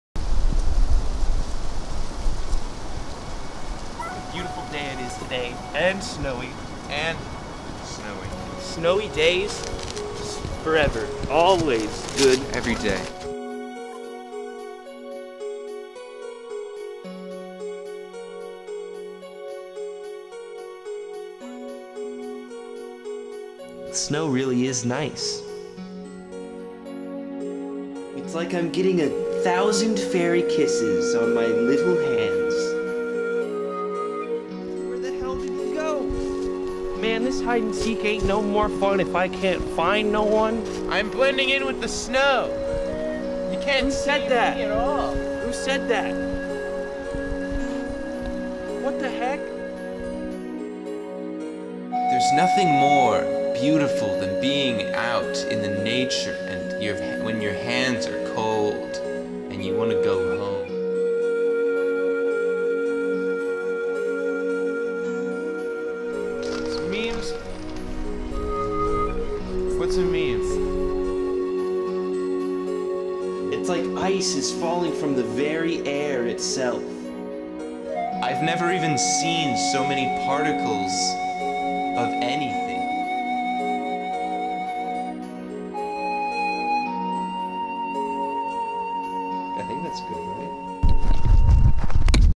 Two Icy Boys: A Nature Documentary
Two icy boyhood men venture into the ice and snow to share their thoughts, ideas, philosophies, and positive vibes with their newfound snow environment. Did I mention it was icy and snowy

Show this to your grandmother